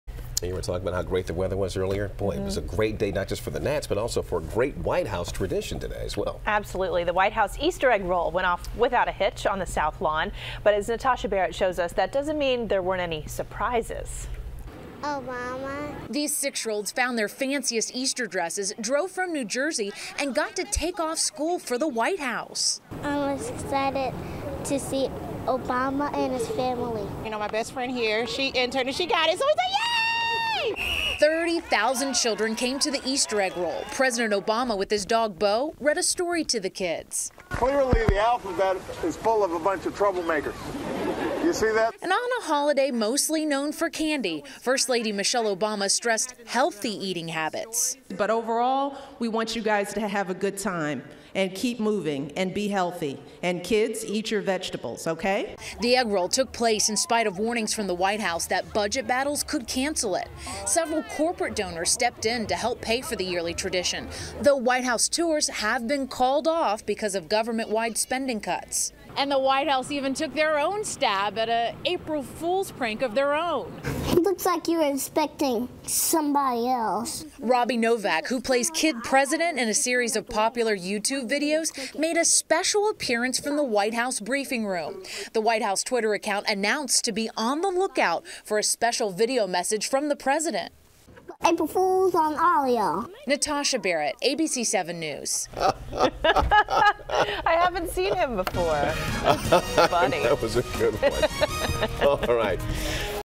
White House Easter Egg Roll brings in celebrities, excited children
President Barack Obama and his family welcomed thousands of children to the White House for the annual Easter Egg Roll.